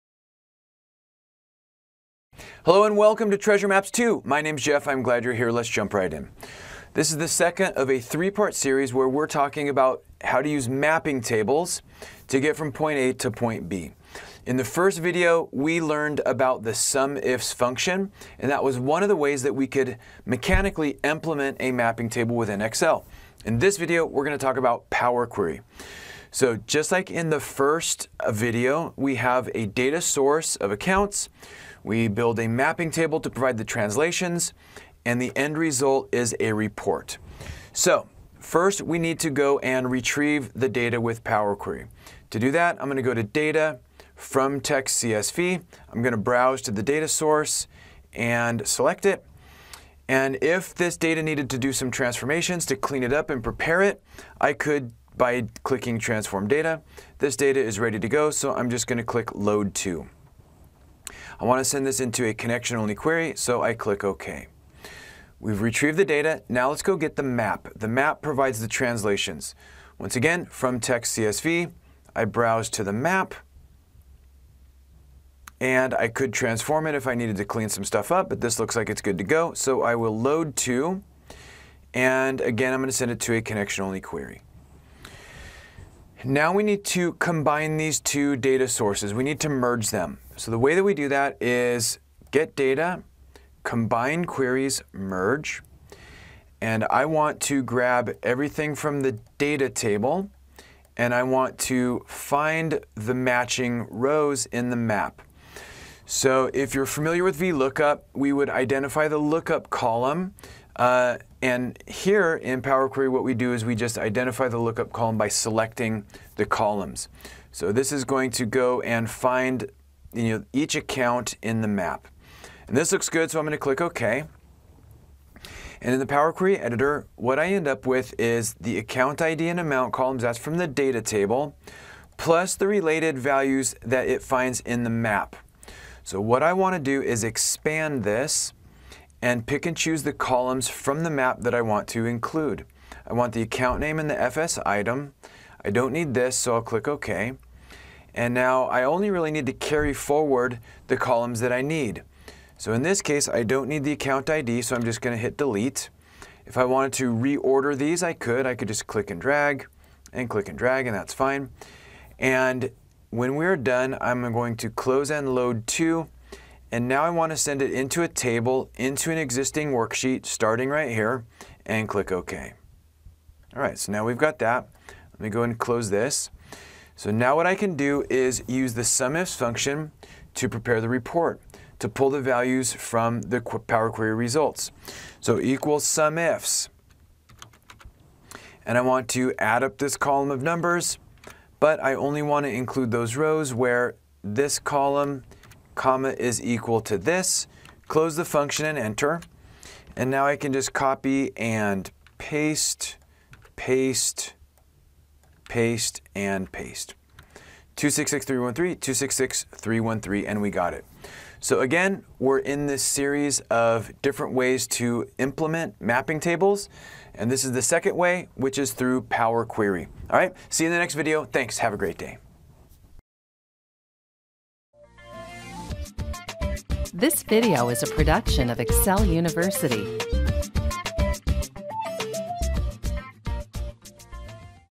Treasure Maps 2
This is the second post in the Treasure Maps series, where we are discussing various ways to implement mapping tables. In the first post, we discussed SUMIFS. In this post, we'll talk about Power Query.